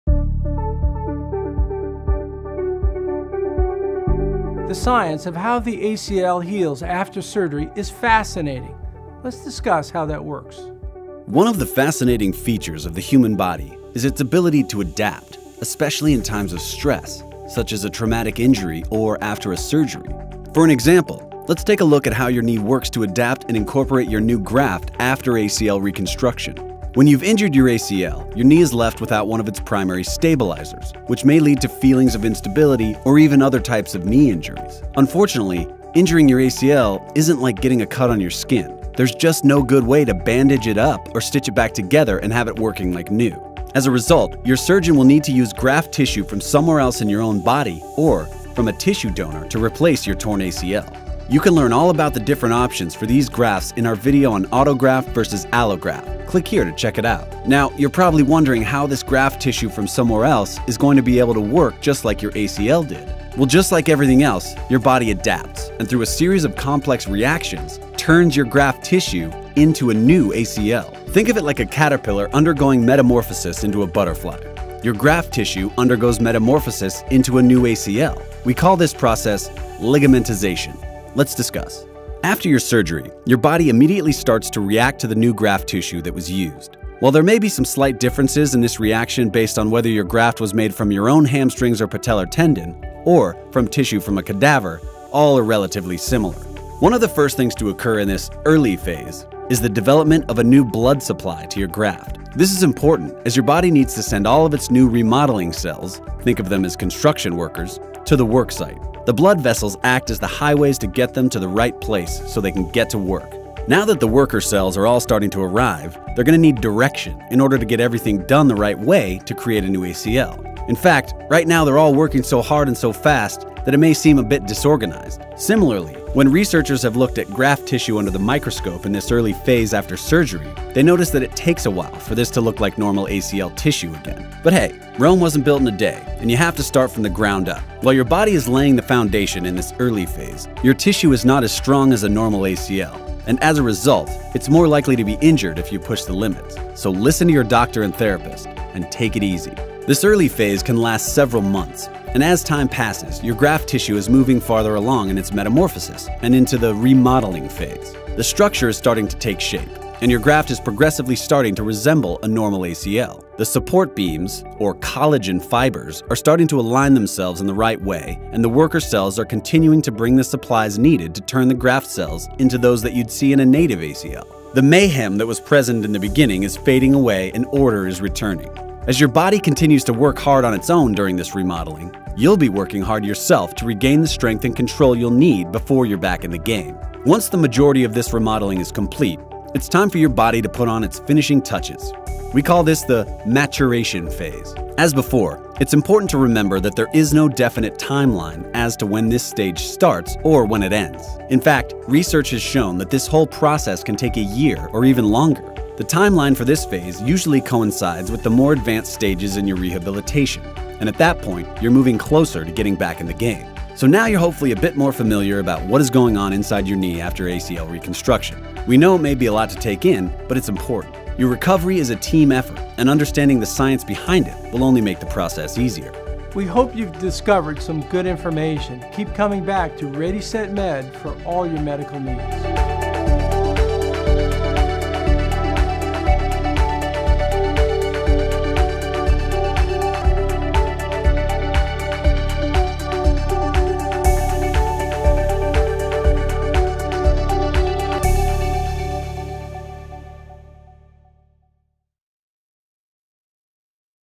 How Does My Knee Heal After ACL Surgery? - ACL Series
Curious about how the healing process works after ACL surgery?  This video will take you through the science of ligamentization, and how your body turns your graft into your new ACL.

ReadySetMed does not provide medical advice, diagnosis or treatment.